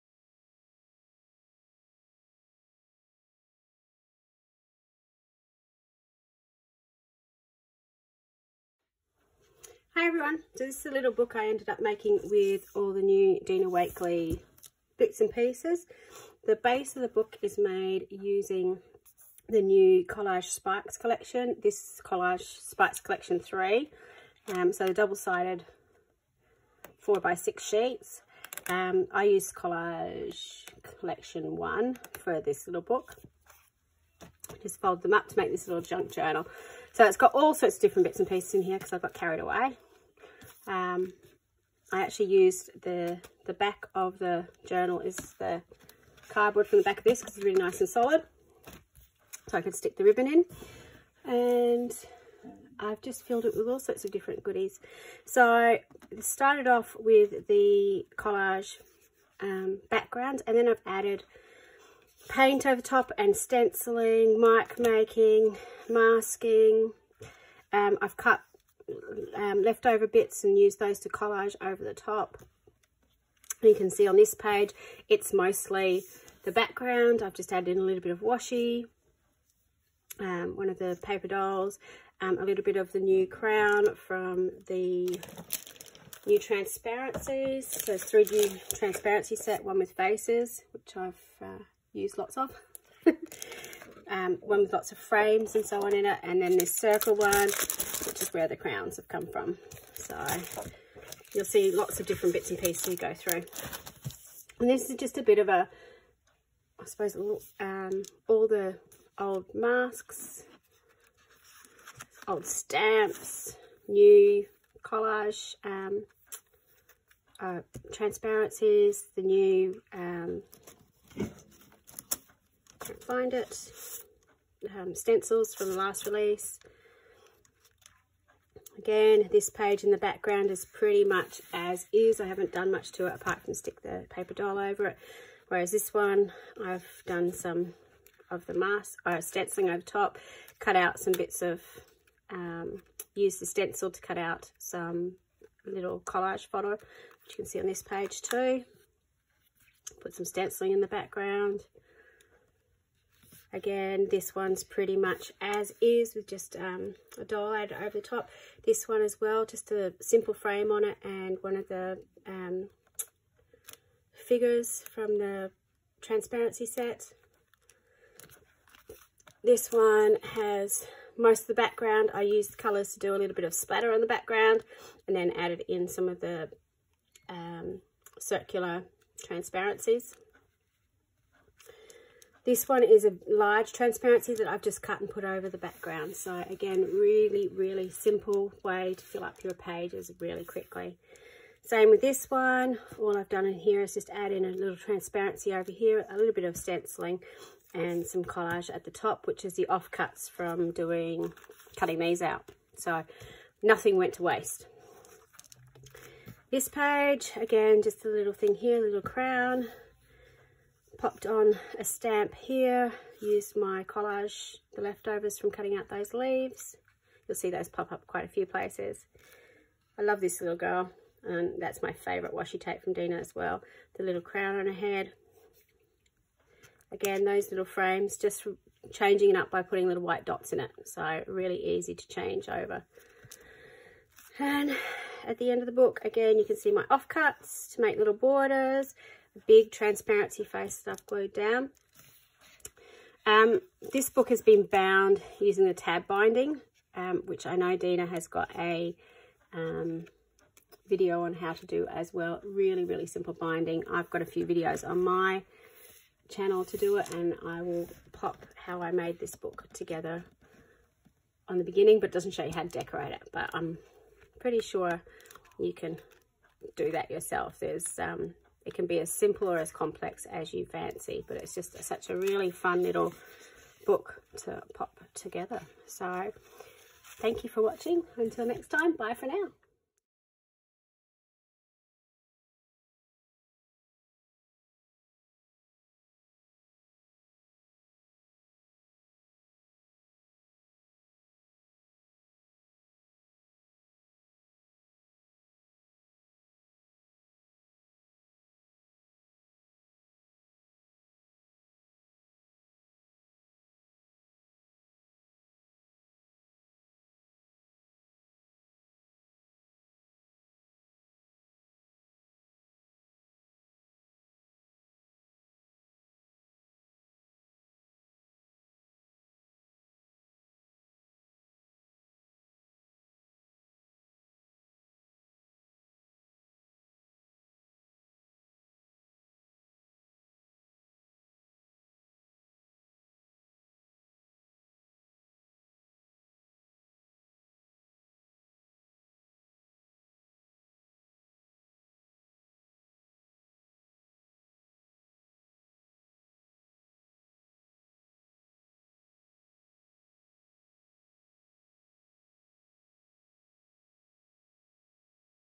Flip through of Collage Sparks Junk Journal
Create a really simple mini junk journal in 15 minutes (less if you can get the backing of the red liner tape quicker than me!!!). Such a fun little project that can be customised really easily.

My collage rice papers designs manufactured by Scrap FX available (wholesale)